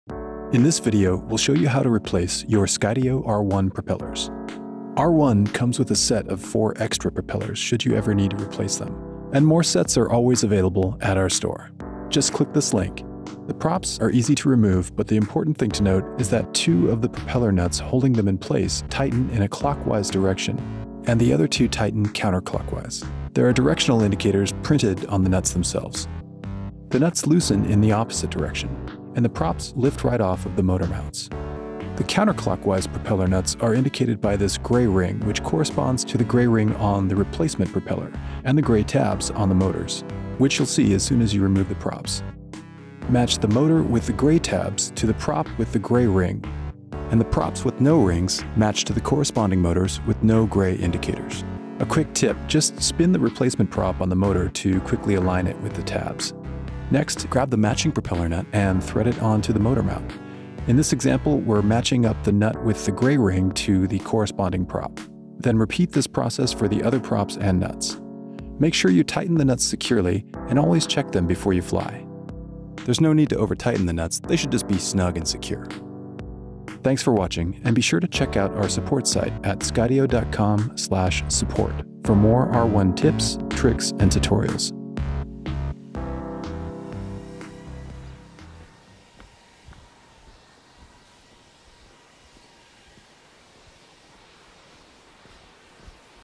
Replacing Skydio R1 Propellers
A quick video tutorial on how to replace the propellers on Skydio R1.